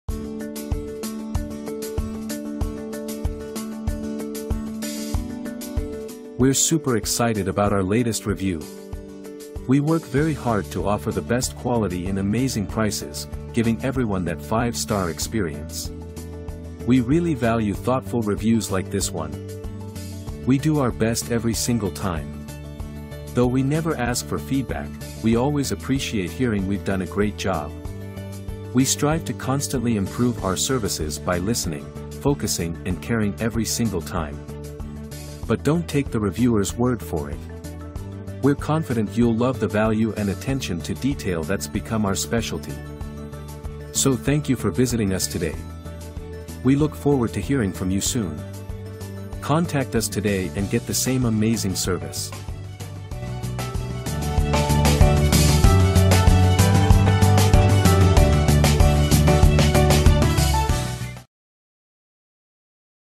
Black Mountain Plumbing Inc San Diego Wonderful 5 Star Review by John White
Excellent Rating

Prompt response, professional workmanship,  and thorough completion.  Outstanding job.

Black Mountain Plumbing Inc
9909 Hibert St
San Diego California
92131